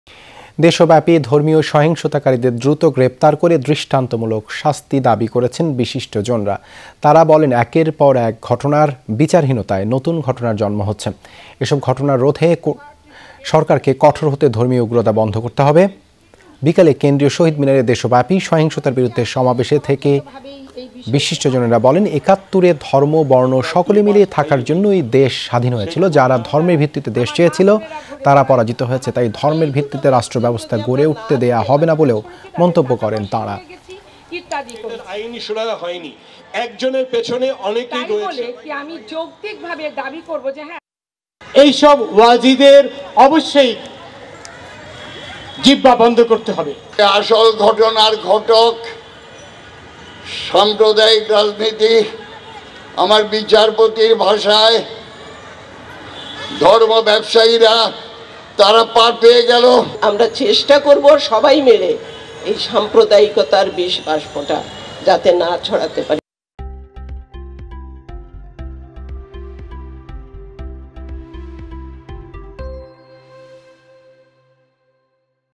ধর্মীয় সহিংসতাকারিদের দৃষ্টান্তমূলক শাস্তি দাবি করেছেন বিশিষ্টজনরা | Protest
দেশব্যাপী ধর্মীয় সহিংসতাকারিদের দ্রুত গ্রেফতার করে দৃষ্টান্তমূলক শাস্তি দাবি করেছেন বিশিষ্টজনরা। তারা বলেন, একের পর ঘটনার বিচারহীনতায় নতুন ঘটনার জন্ম হচ্ছে। এসব ঘটনা রোধ করতে হলে সরকারকে কঠোর হাতে ধর্মীয় উগ্রতা বন্ধ করতে হবে। বিকালে কেন্দ্রীয় শহীদ মিনারে দেশব্যাপী সহিংসতার বিরুদ্ধে সমাবেশে থেকে বিশিষ্টজনরা বলেন, একাত্তরে ধর্ম-বর্ণ সকলে মিলে থাকার জন্যই দেশ স্বাধীন হয়েছিল। যারা ধর্মের ভিত্তিতে দেশ চেয়েছিল তারা পরাজিত হয়েছে। তাই ধর্মের ভিত্তিতে রাষ্ট্র ব্যবস্থা গড়ে উঠতে দেয়া হবে না বলেও মন্তব্য করেন তাঁরা।